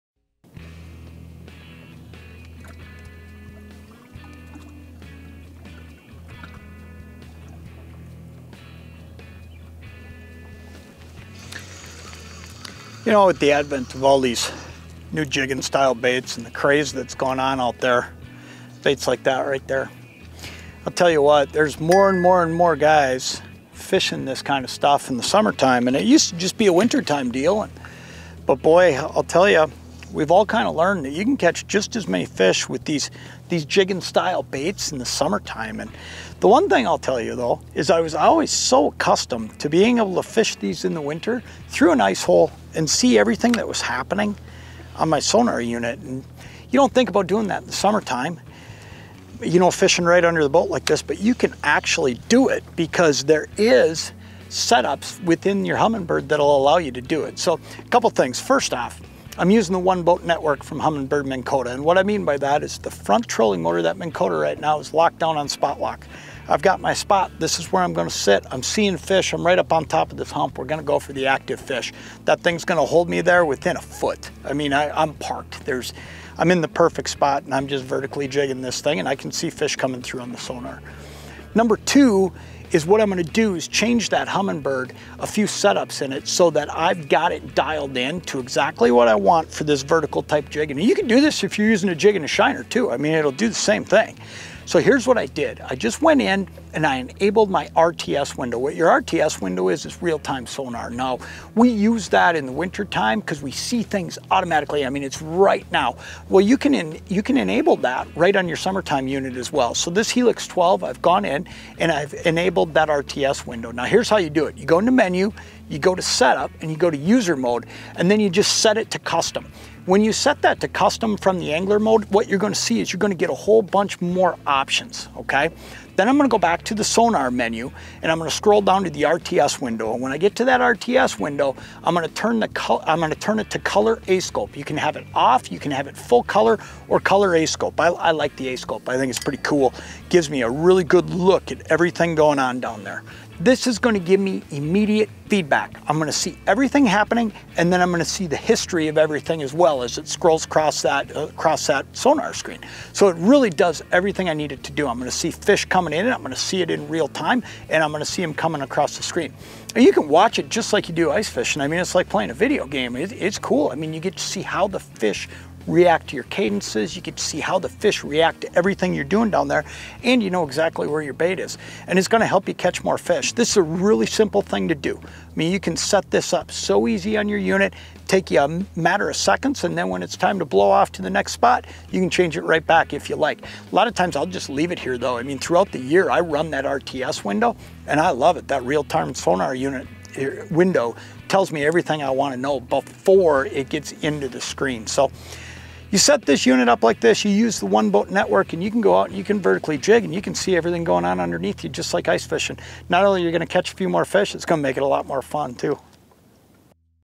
Fishing Vertically With Real Time Sonar
Jon Thelen talks about fishing jigging style baits vertically with the use of Humminbird's "Real Time" sonar function and how to set the unit up to do it.